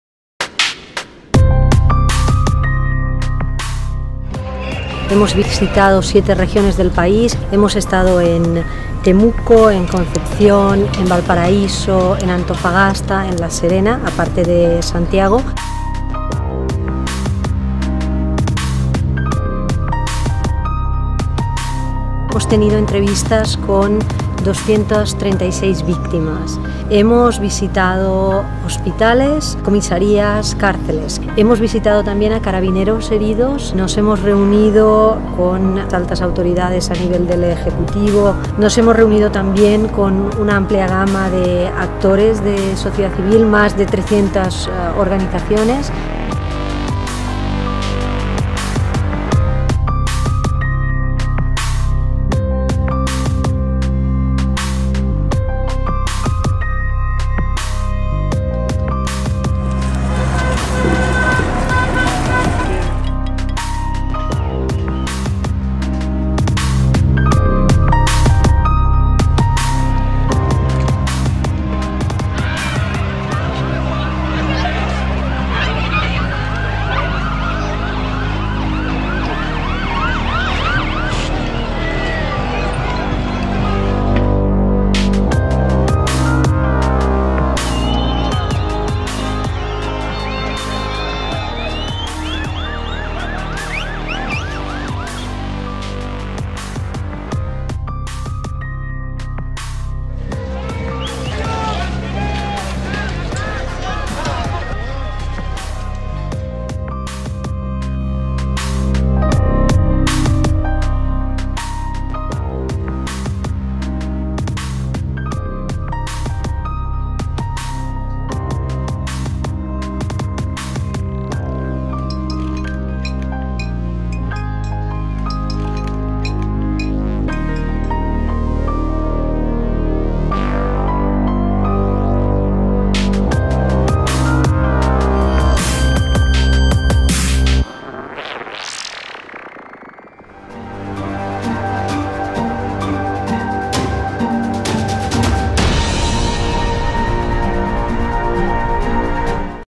Informe ONU sobre violaciones de DDHH en Chile
Informe de @ONU_derechos sobre protestas y estado de emergencia en Chile concluyó que durante la crisis política y social ha habido graves hechos de violación de DDHH, señalando que Carabineros y el Ejército incumplieron las normas y estándares internacionales sobre control de asambleas y uso de la fuerza, y reclama reformas.